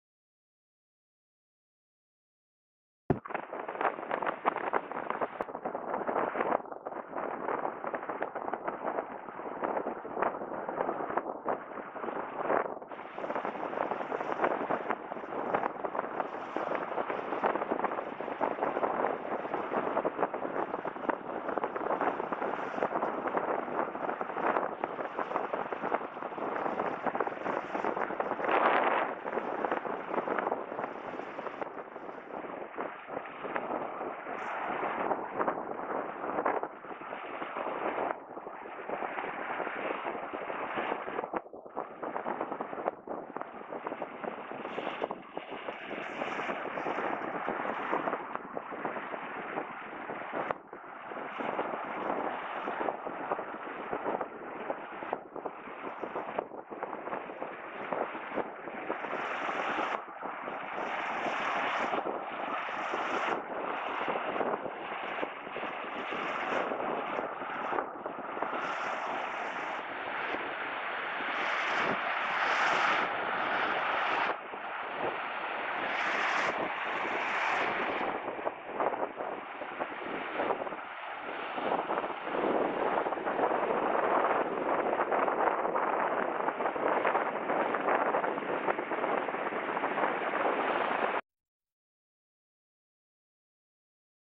Chemie- und Öl Produkt Tanker BALTIC SKY I ++NORIENT++
Diesmal ist der kombinierte Chemie- und Öl Produkt Tanker BALTIC SKY I von NORIENT zu sehen, der vom Chemie/Öl-Terminal Antwerpen kommt und hier gerade an den  Stränden von Zoutelande und Westkapelle entlang fährt und in Fahrtrichtung offene Nordsee unterwegs ist.